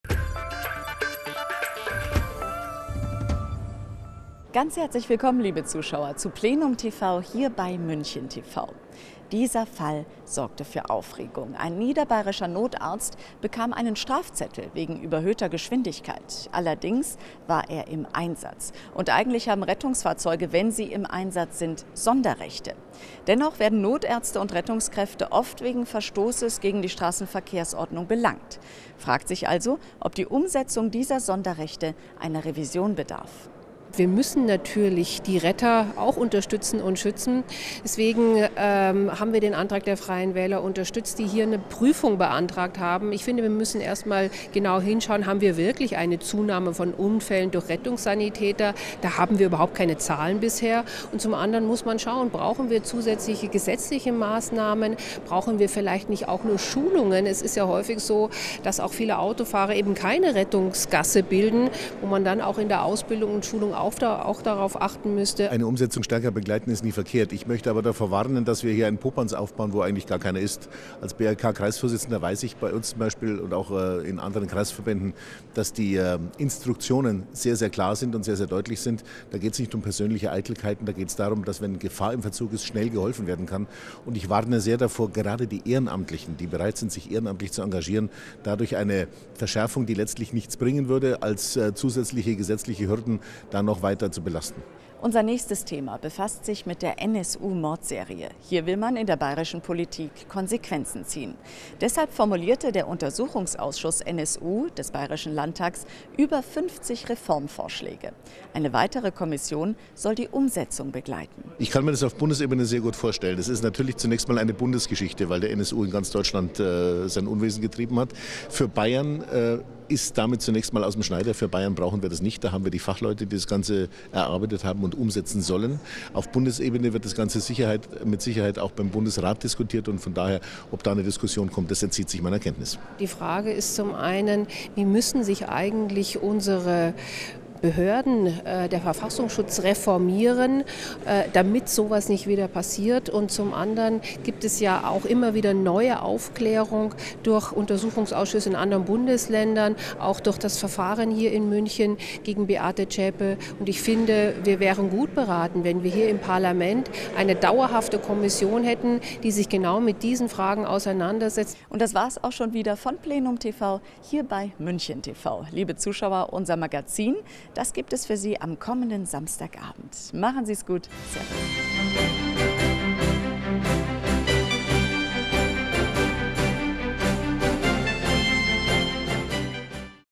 Plenum.TV für München am 23.04.2015
Themen: Sonderrechte für Rettungskräfte & NSU-Reformen
Redaktion: Uwe Brückner, Maximilian von Rossek, Andreas Matsche